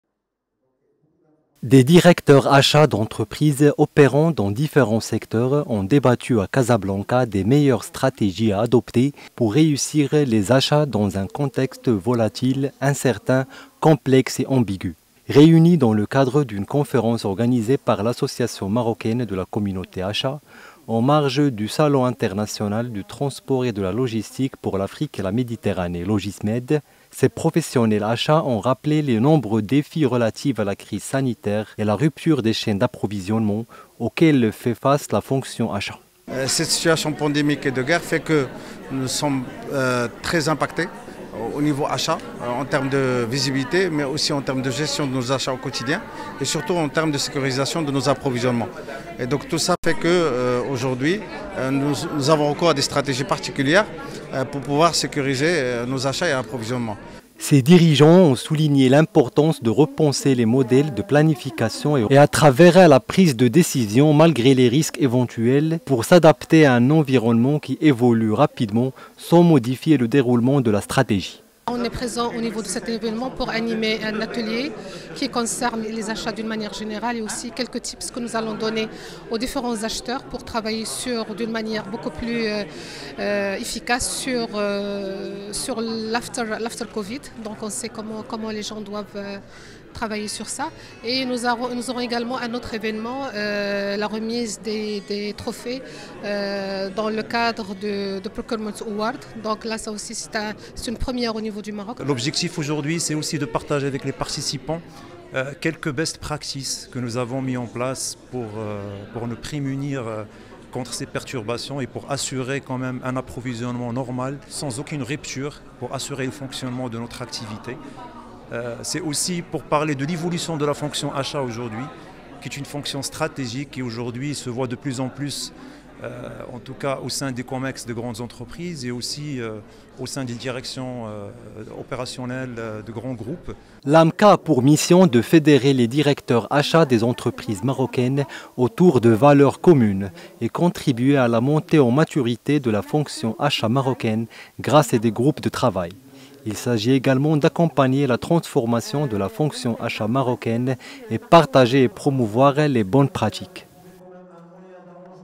Logismed 2022: Débat sur les meilleures stratégies Achats dans un contexte VUCA
Des directeurs Achats d'entreprises opérant dans différents secteurs ont débattu, mercredi à Casablanca, des meilleures stratégies à adopter pour sécuriser les achats dans un contexte Volatil, Incertain, Complexe et Ambigu (VUCA).